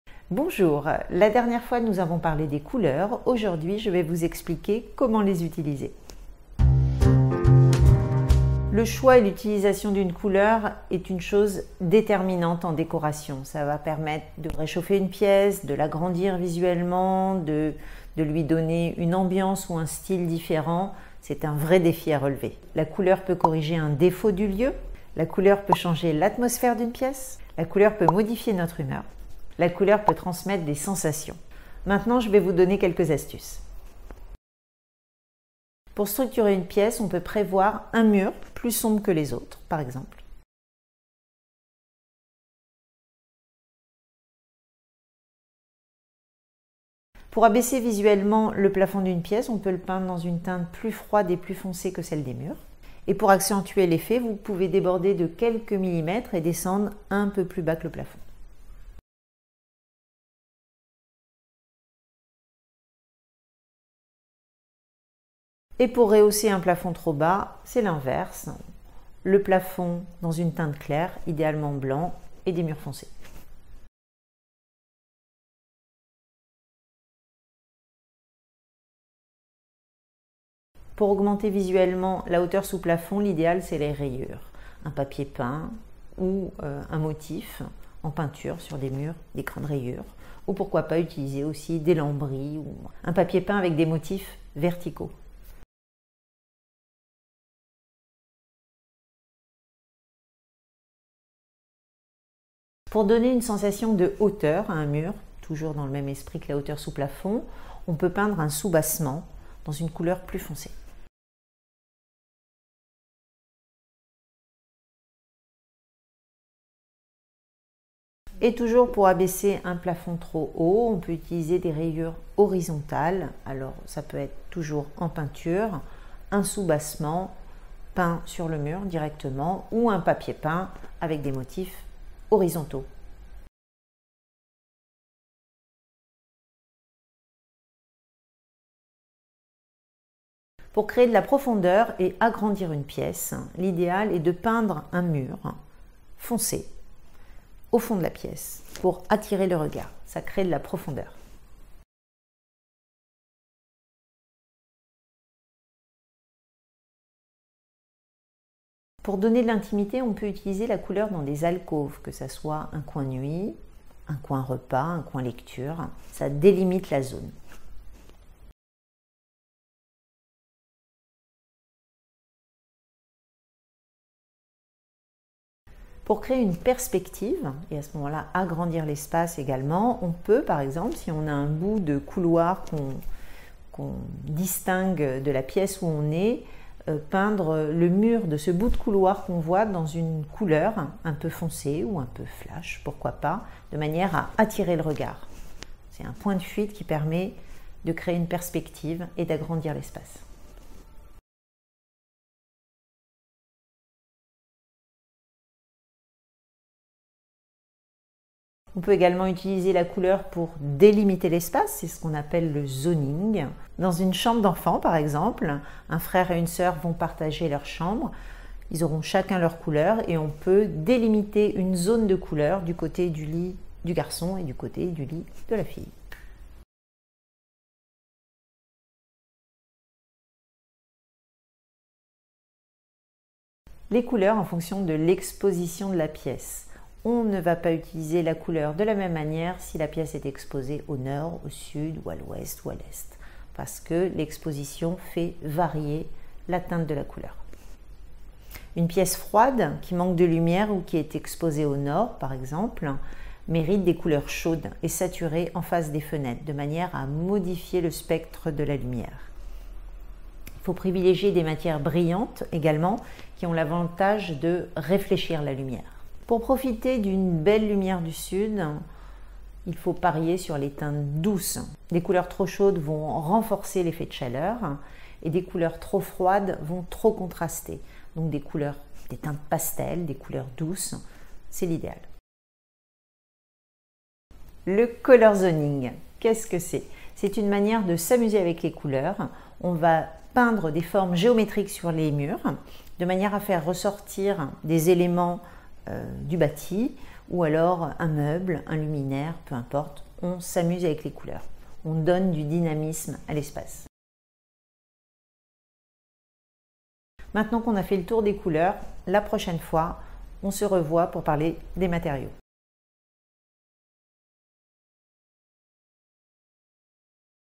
Quel mur peindre pour agrandir sa pièce : nos astuces pour bien travailler les espaces
Profitez des conseils de peinture de notre formatrice en décoration d'intérieur pour égayer vos pièces de vie. Élargir son salon, augmenter la hauteur sous plafond de sa chambre ou raccourcir un couloir, tout est possible grâce à une bonne utilisation des couleurs.

Découvrez quel mur peindre en couleur pour quel effet et faites passer votre décoration d'intérieur au niveau supérieur.
En attirant l’attention et en captant le regard, les couleurs permettent de corriger les volumes et d’en changer la perception : elles agrandissent ou rapetissent une pièce, rapprochent ou éloignent les murs. Elles sont aussi capables d’atténuer des défauts de proportions, de modifier la sensation d’espace.

Dès la fin de cette vidéo, vous serez capable de choisir la bonne couleur pour peindre vos murs.

Vous souhaitez en savoir plus ? Découvrez notre formation en décoration d'intérieur 100% en ligne et éligible au CPF.